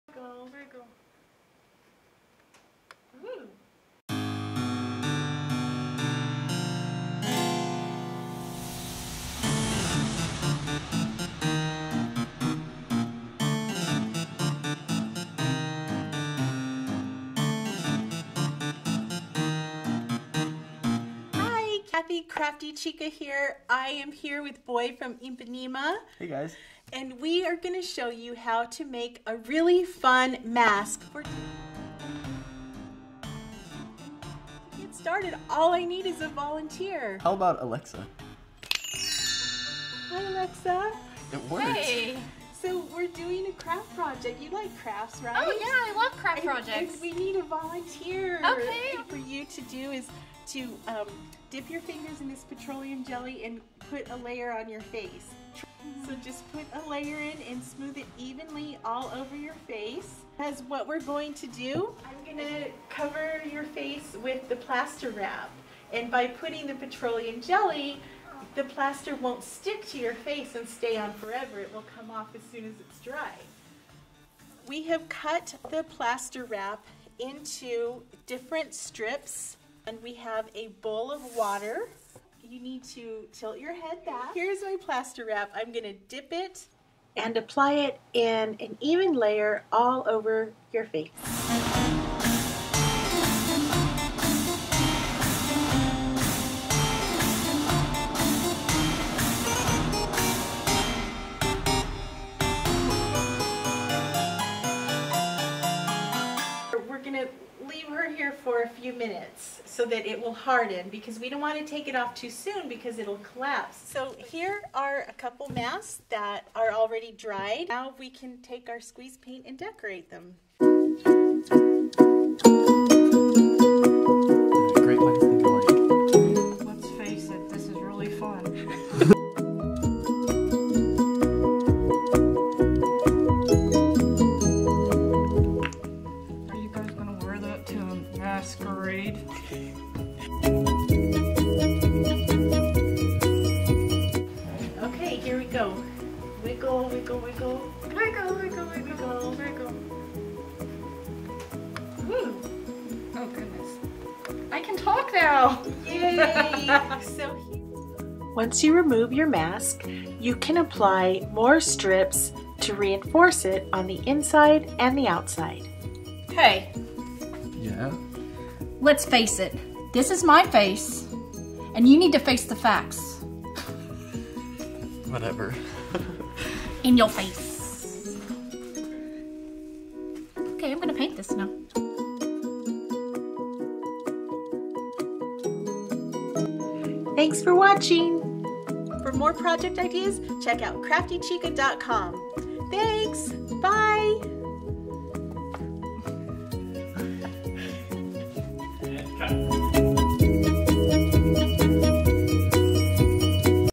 HOW TO: Make a Creepy Plaster Mask for Halloween
Alexa and Jeshua stop by to help me make a crazy looking mask for Halloween using plaster wrap. Music used by permission from Audio-Micro.